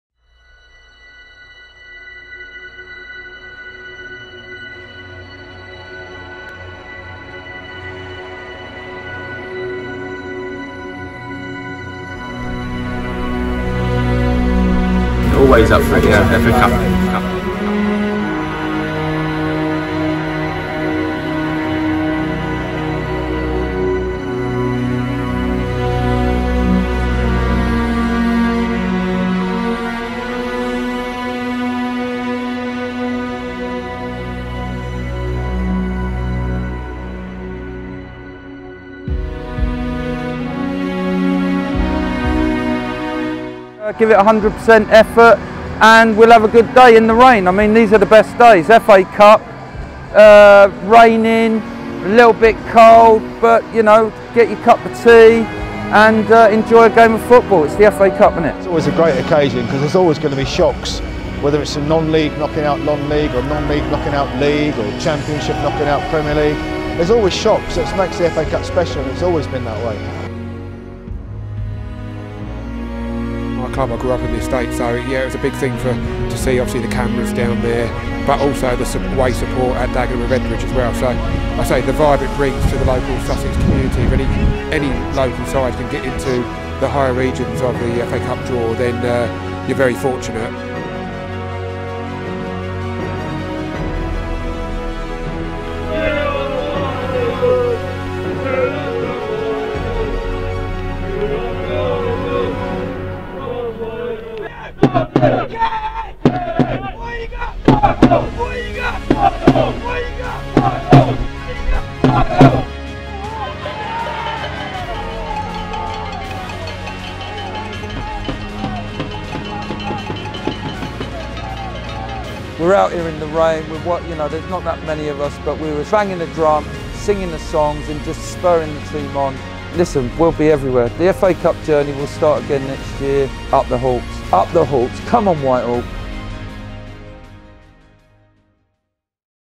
Power of the Cup | Whitehawk FC
Our Power of the Cup series begins as we followed Whitehawk FC on their FA Cup journey.